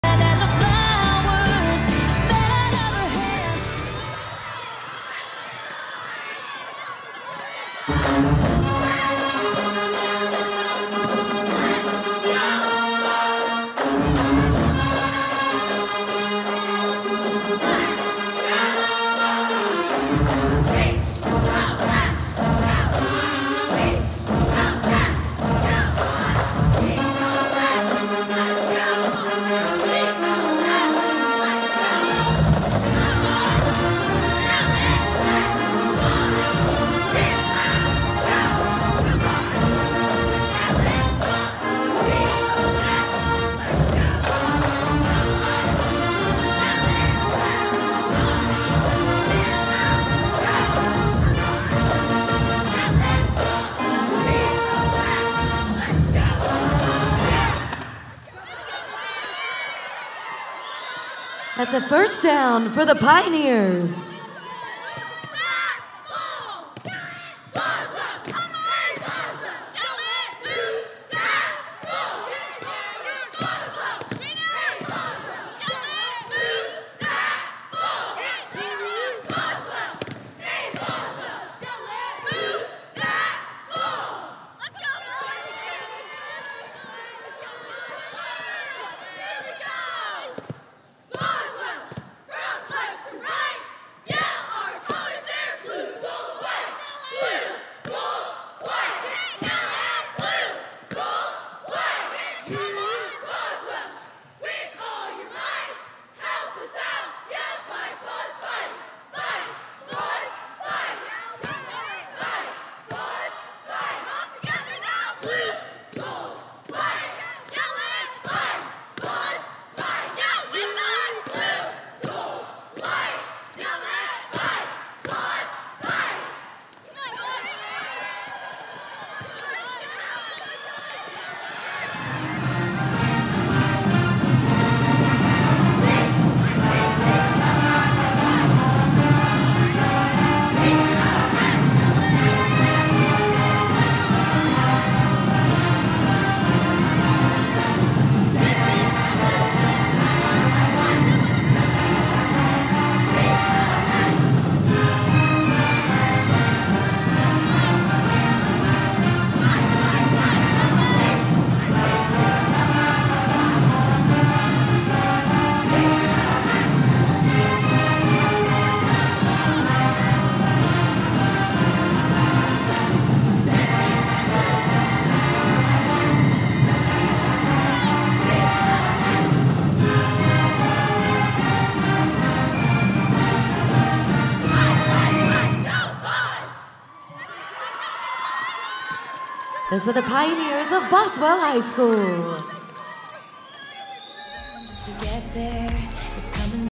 Boswell High School (prelims) 2-9-24
2024 UCA - NHSCC in Orlando
Varsity Coed NT D1 GD
2/9/24–2/11/24

Universal Cheer Association
National High School Cheerleading Championship
in Orlando, Florida
Varsity Coed   Non-Tumbling   Division 1   Game Day

The video is a performance of cheer/dance choreography which is enhanced by the accompanying or background music.